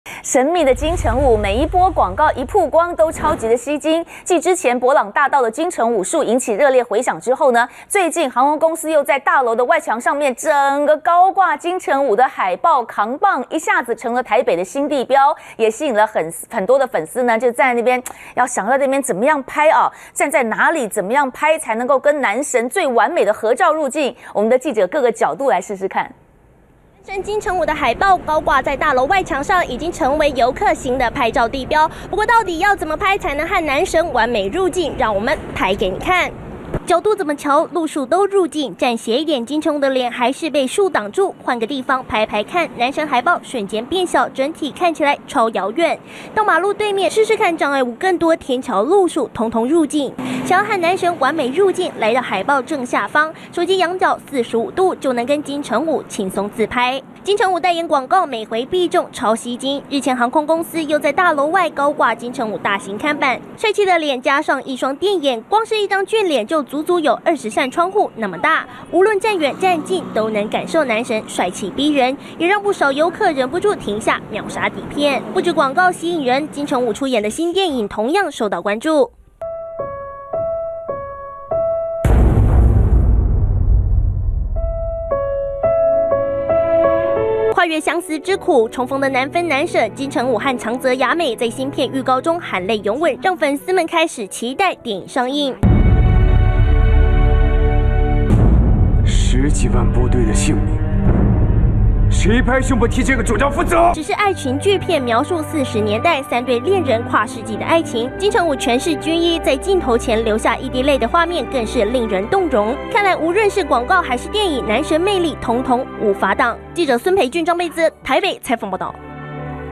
20141107中天新聞 金城武帥照高掛大樓 如何合照報你知
男神金城武每一波廣告曝光都超極吸睛！繼伯朗大道「金城武樹」引起熱烈迴響後，近日航空公司又在大樓外牆高掛金城武海報，看板穿著皮衣的金城武一雙電眼望向天空，頓時成了台北新地標，當然也因此吸引不少粉絲駐足拍照，不過到底要站在哪裡？要怎麼拍才能跟男神完美合照入鏡，帶您一探究竟。